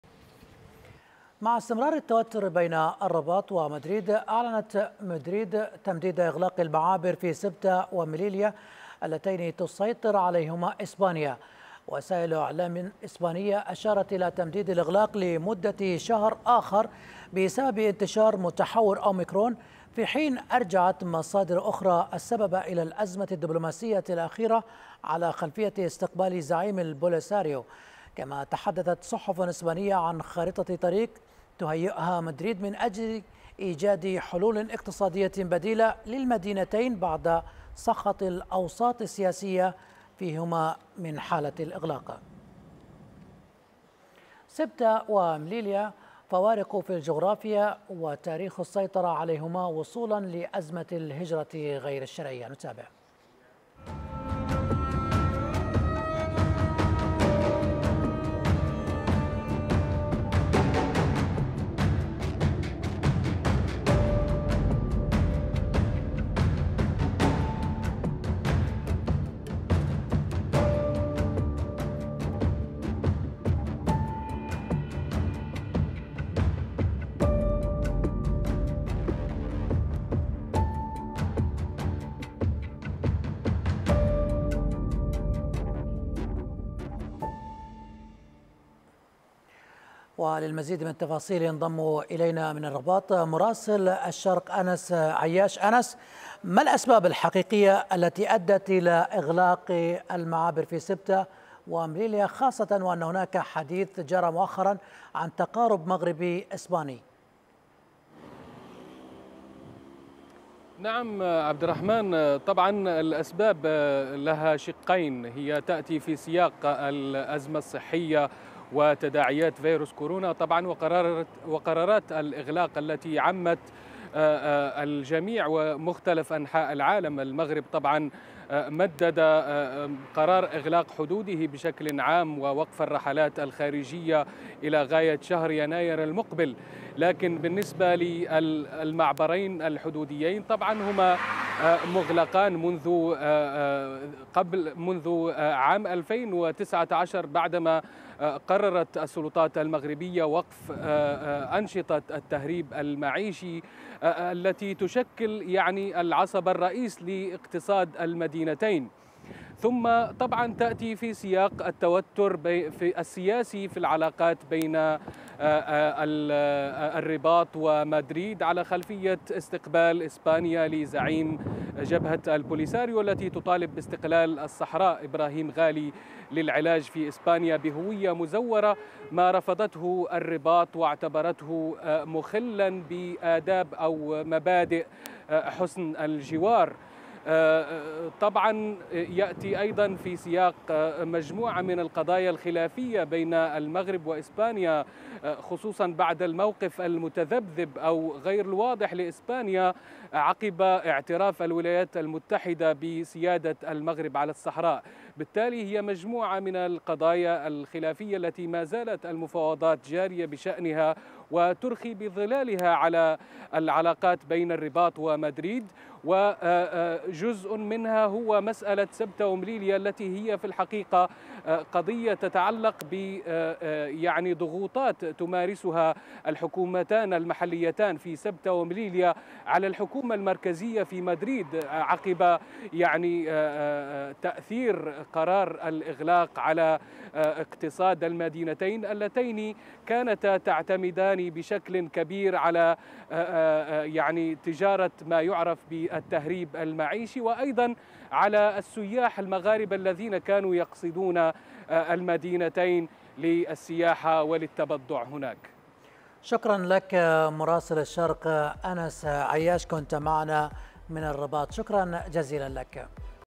إسبانيا تمدد إغلاق معبري سبتة ومليلية المغربيين - أخبار الشرق
مع استمرار التوتر بين المغرب وإسبانيا، أعلنت مدريد تمديد إغلاق المعابر في سبتة ومليلية الذين تسطير عليهما إسبانيا، وسائل إعلام إسبانية أشارت إلى تمديد الإغلاق لمدة شهر آخر بسبب انتشار متحور أوميكرون، في حين أرجعت مصادر أخرى السبب إلى الأزمة الدبلوماسية الأخيرة على خلفية استقبال زعيم البوليساريو